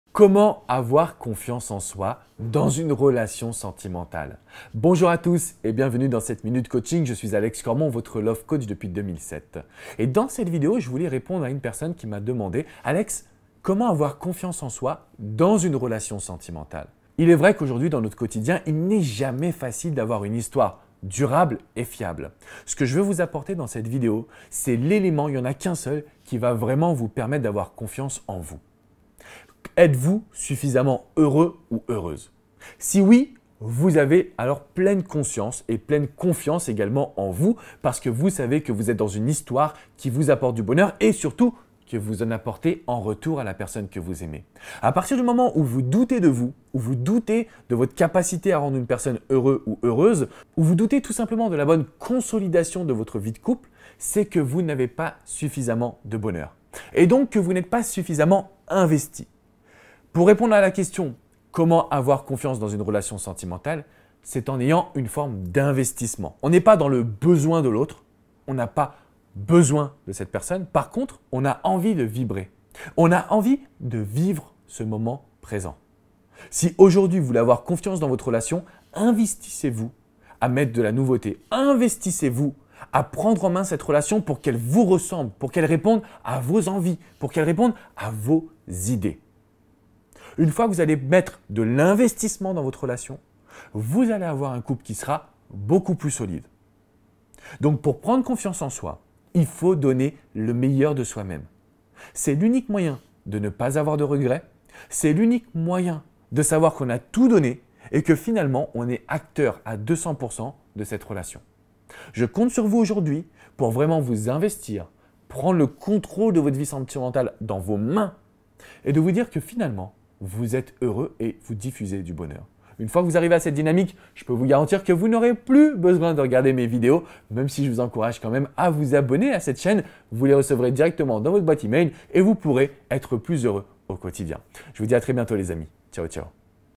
Comment avoir confiance en soi en Amour ?
Comment avoir confiance en soi en Amour ? Comment avoir une vie sentimentale heureuse et épanouissante ?

Dans cette vidéo je vous aide à avoir plus confiance pour obtenir du bonheur et du bien être dans votre vie sentimentale.

Il est important de pouvoir se sentir "soi-même" et de prendre confiance pour mettre en avant sa personnalité. Vous devrez dès aujourd'hui laisser parler vos envies pour construire la vie sentimentale de VOS rêves !

Si vous avez la moindre question n'hésitez pas à me la poser dans les commentaires.

Amitiés
Alex

Pour cela n'hésitez pas à venir consulter mon ou me suivre sur mes différents comptes sociaux